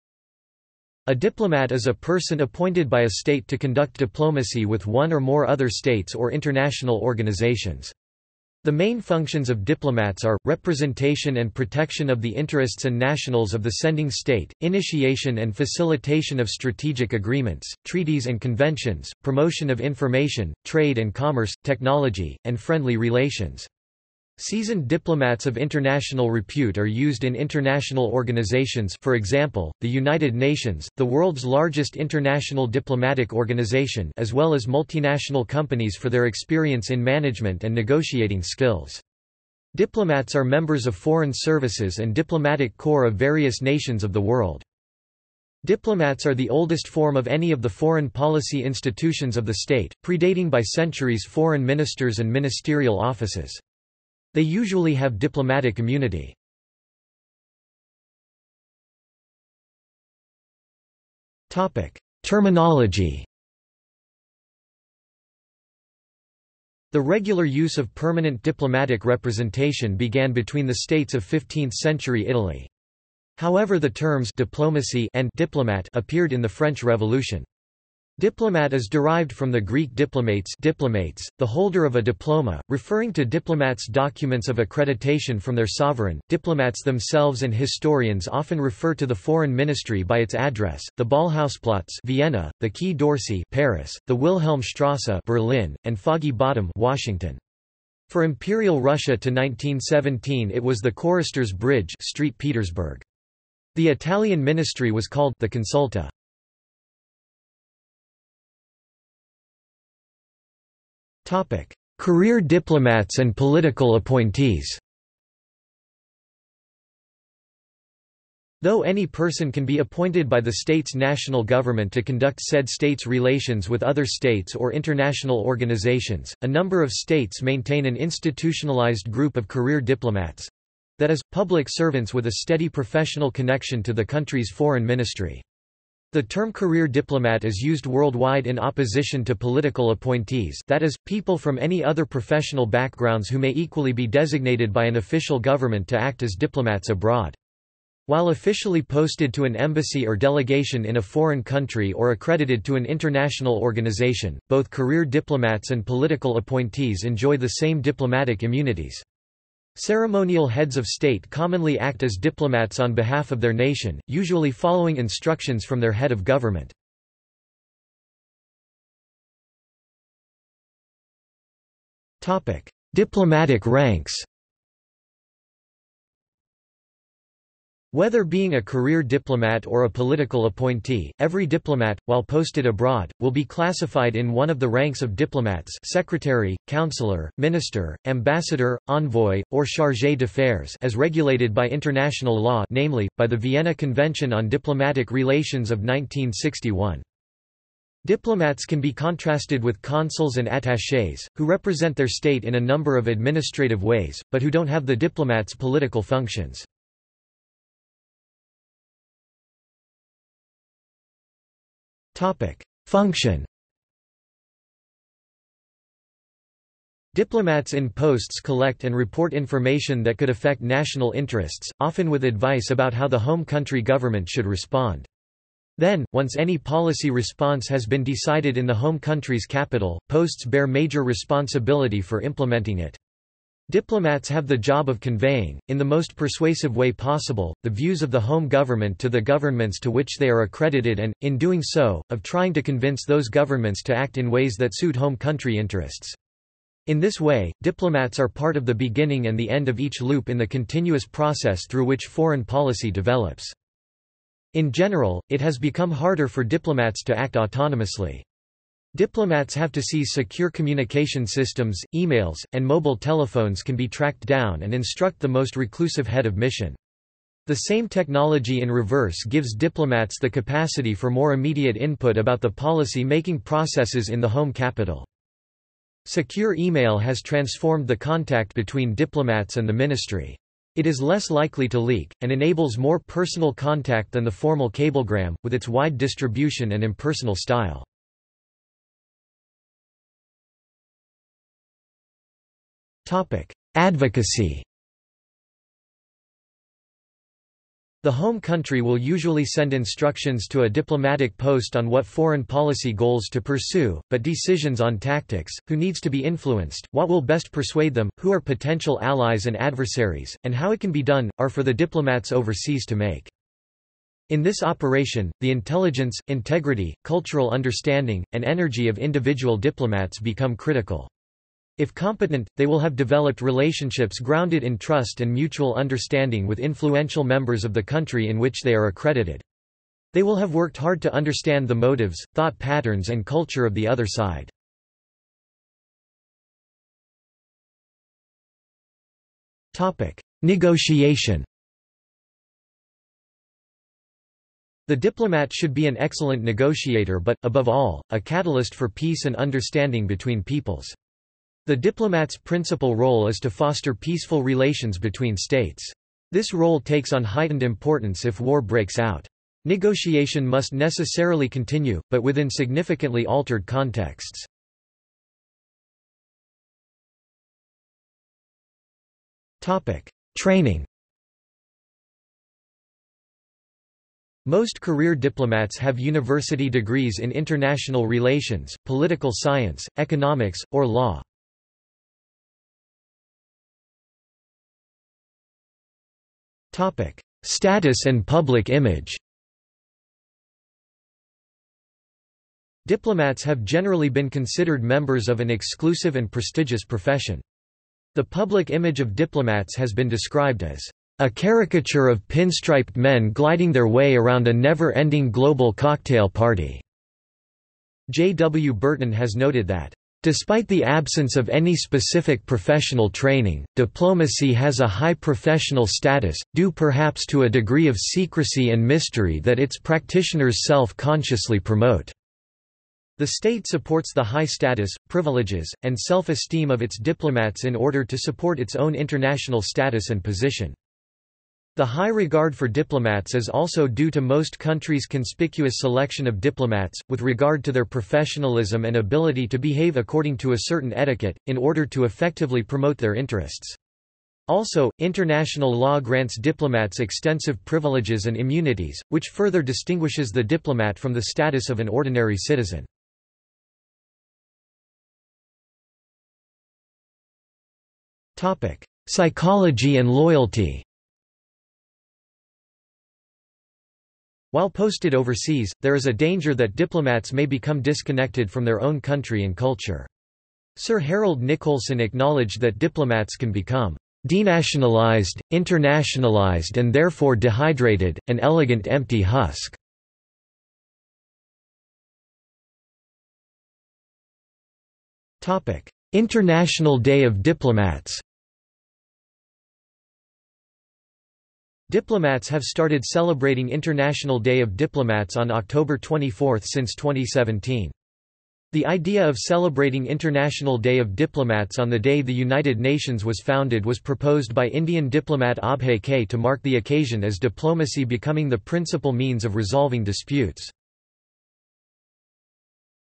Diplomat | Wikipedia audio article
This is an audio version of the Wikipedia Article:
Diplomat

00:00:57 1 Terminology
00:01:47 2 Career diplomats and political appointees
00:02:48 3 Diplomatic ranks
00:03:28 4 Function
00:04:49 4.1 Advocacy
00:05:36 4.2 Negotiation
00:06:06 5 Training
00:06:23 6 Status and public image
00:07:34 7 Psychology and loyalty
00:08:01 8 International Day of Diplomats

- Socrates

SUMMARY
A diplomat is a person appointed by a state to conduct diplomacy with one or more other states or international organizations. The main functions of diplomats are: representation and protection of the interests and nationals of the sending state; initiation and facilitation of strategic agreements; treaties and conventions; promotion of information; trade and commerce; technology; and friendly relations. Seasoned diplomats of international repute are used in international organizations (for example, the United Nations, the world's largest international diplomatic organization) as well as multinational companies for their experience in management and negotiating skills. Diplomats are members of foreign services and diplomatic corps of various nations of the world.
Diplomats are the oldest form of any of the foreign policy institutions of the state, predating by centuries foreign ministers and ministerial offices. They usually have diplomatic immunity.